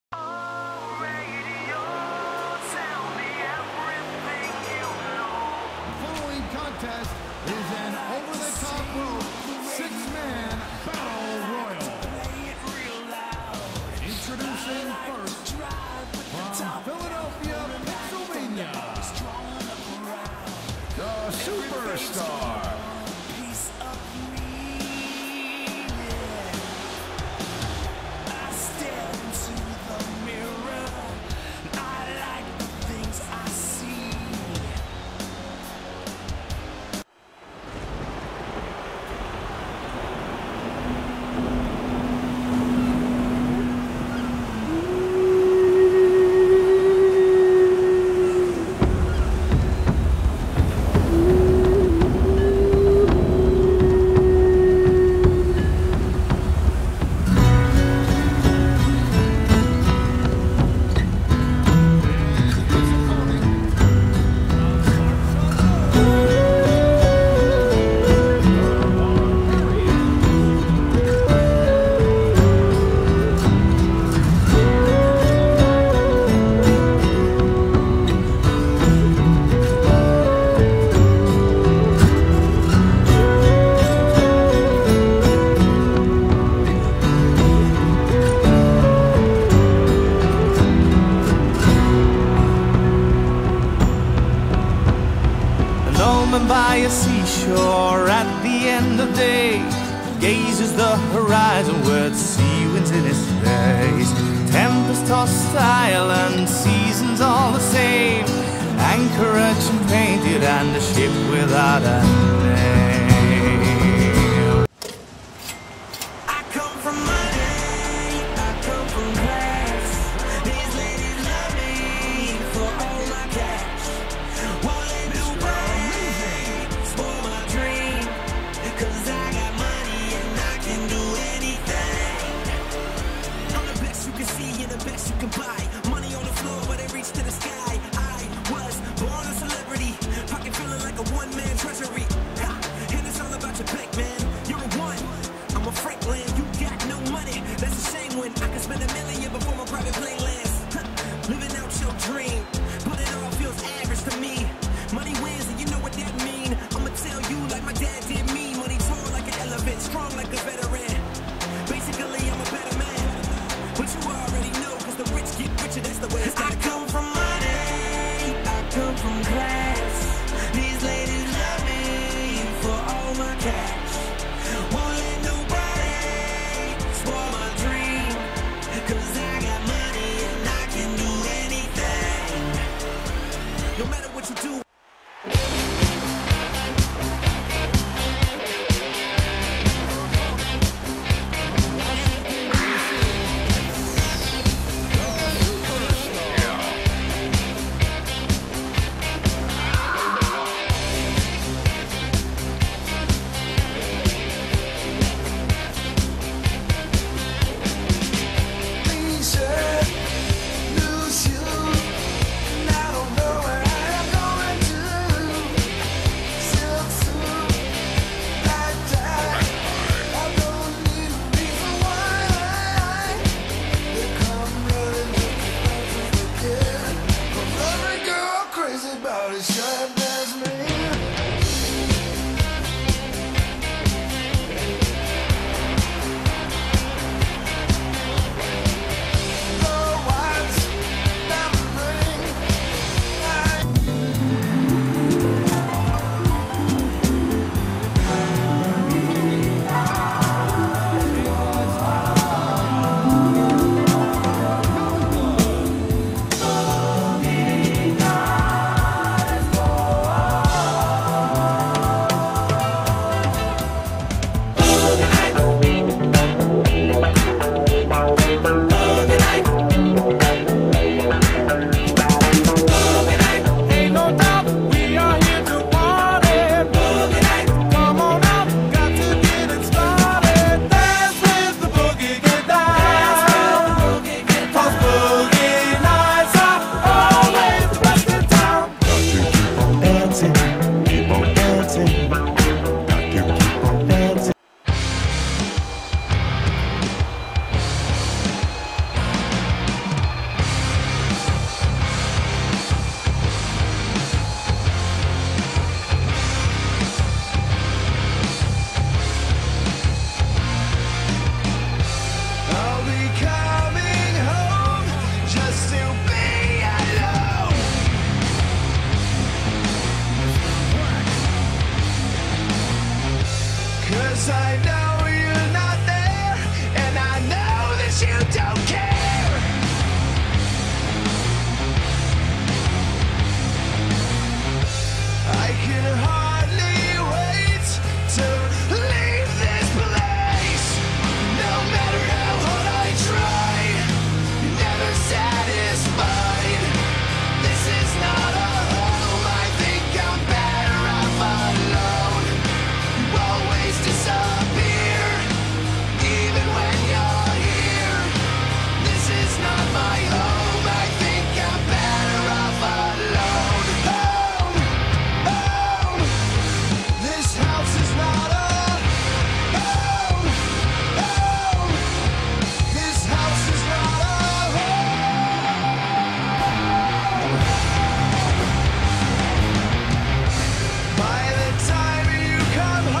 SOT Countdown Video 2
Ty Q- Yeah...sorry Socko (Socko105's brother)

"The Man of the Sea" Captain John Wolfre- He was lost at sea in early adult life...30 years later he's in SOT...WTF

"Mr. Money" Daniel Fonto- Rookie wrestler enters lottery. What? He WON?!? The result...

Carl Cordale- Surprise former TWA fans!

"Hollywood" Patrick Tyrani- Once again...surprise former TWA fans!

Emo Xtreme- Yeah...another emo. Pretty much this guy's life sucked and we were able to obtain him because it was his time to be released from the psycho house...